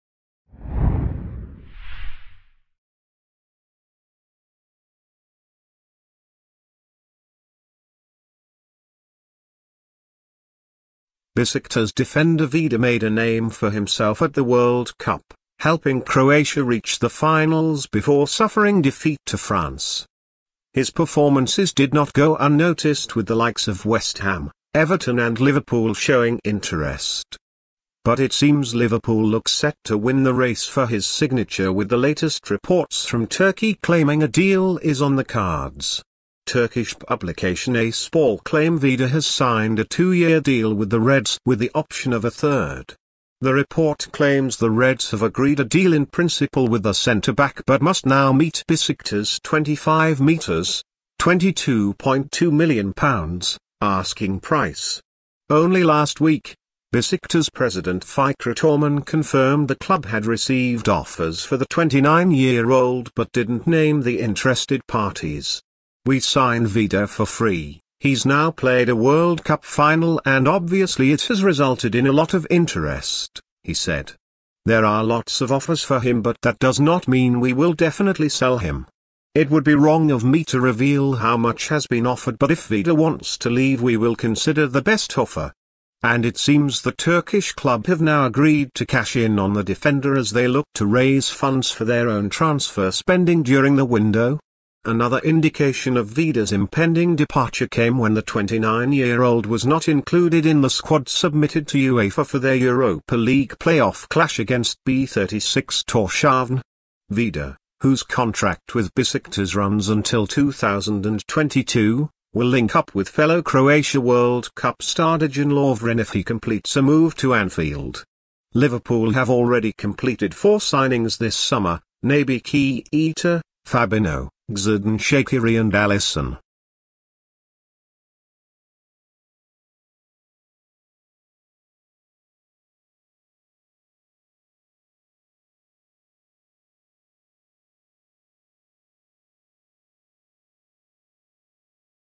Liverpool news: Reds land Domagoj Vida agreement, two-year deal ‘signed’ - report
LIVERPOOL have reportedly reached an agreement in principle for the transfer of Domagoj Vida.
Besiktas defender Vida made a name for himself at the World Cup, helping Croatia reach the finals before suffering defeat to France.
His performances did not go unnoticed with the likes of West Ham, Everton and Liverpool showing interest.